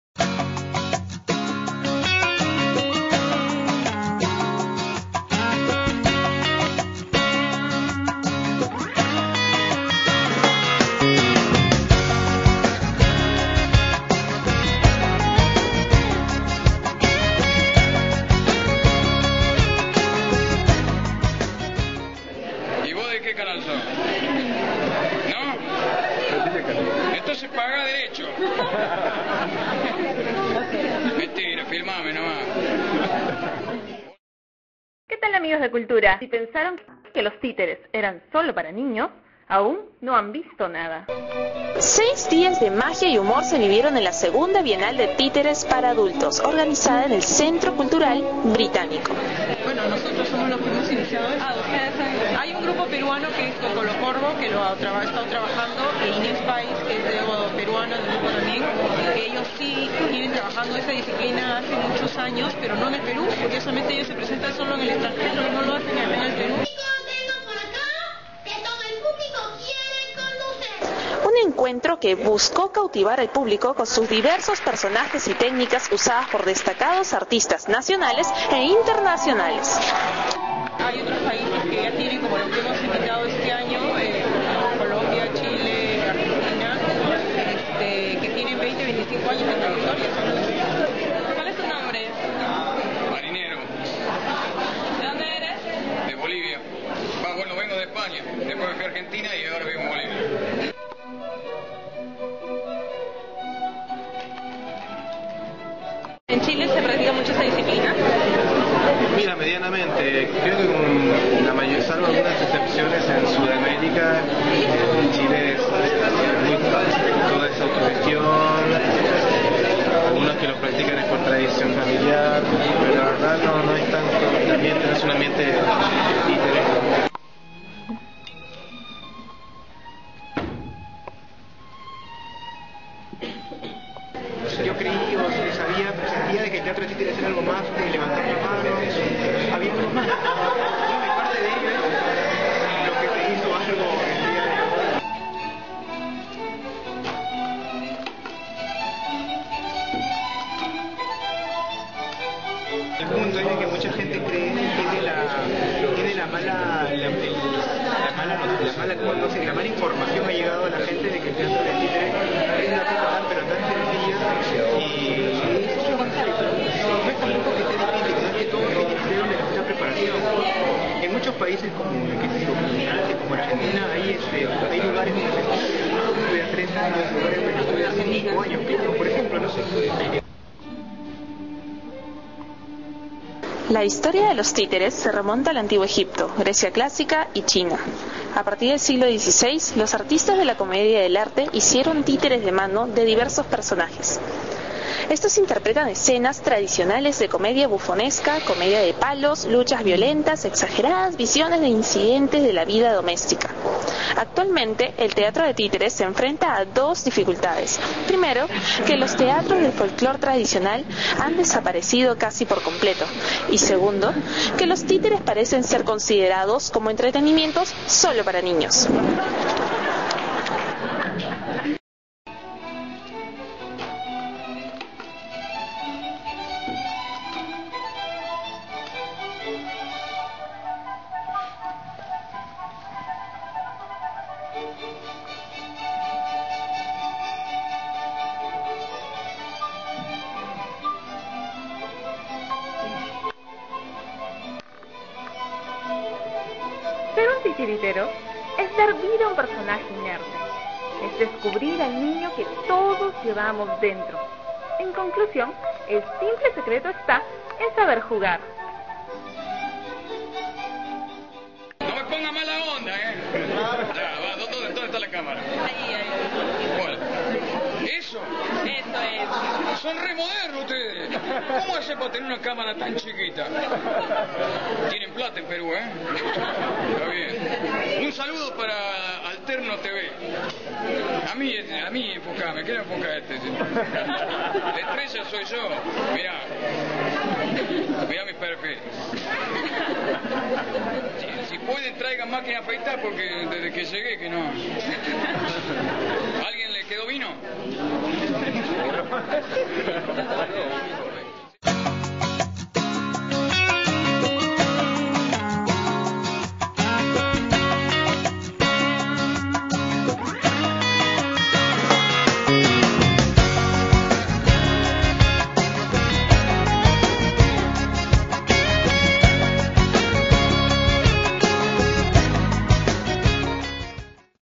Cooltura - II Bienal de títeres para adultos
Una experiencia inolvidable junto a simpáticos personajes que cobraron vida para brindarnos un festival de lujo en el Centro Cultural Británico